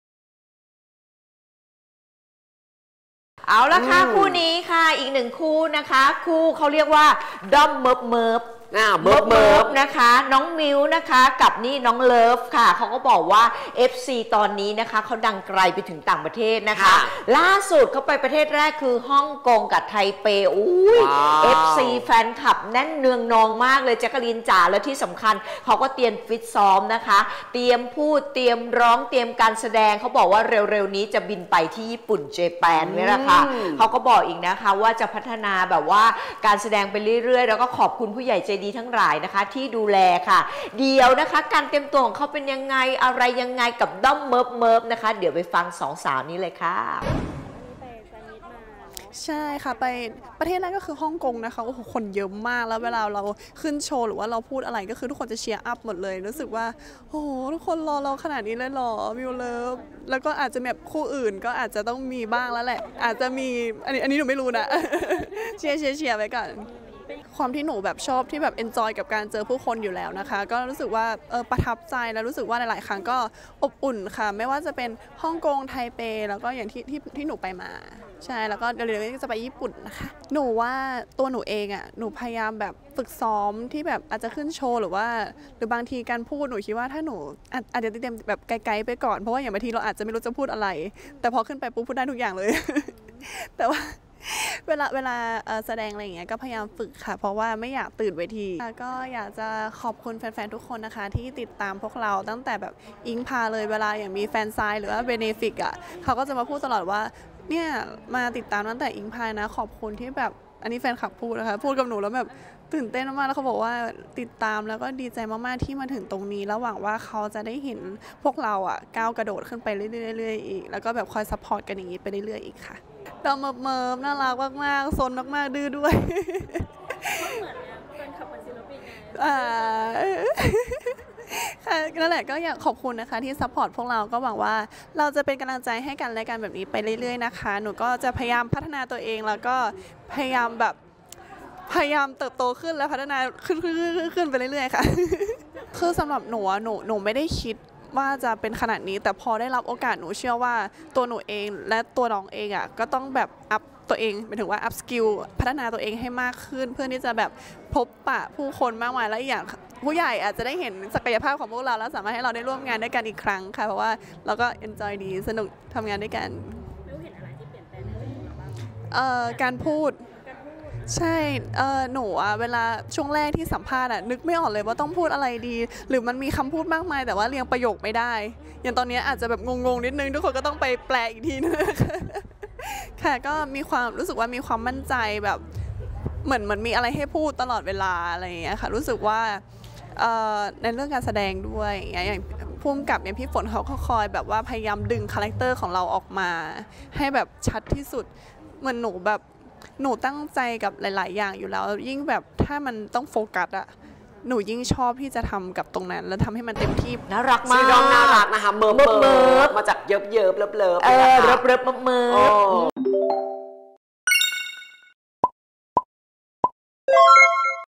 มิลค์ ประทับใจแฟนอินเตอร์ ขอบคุณที่ติดตามกัน I MAYAENTERTAIN
“มิลค์-พรรษา วอสเบียน” เผยถึงการได้ไปแฟนมีตติ้งต่างประเทศคู่กับ “เลิฟ ภัทรานิษฐ์” ประเทศแรกคือ “ฮ่องกง” คนมาเยอะมาก ทุกคนรอมิลค์เลิฟเยอะมากเลย ได้เจอแฟนๆ ก็รู้สึกประทับใจ อบอุ่นมาก ทั้งฮ่องกง ไทเป เร็วๆ นี้จะไปญี่ปุ่น